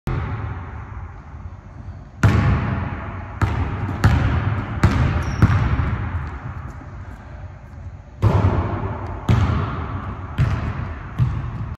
Almost Hung It From Half
I did hang it from half today but not on video.  This was almost half court today so I'll count it.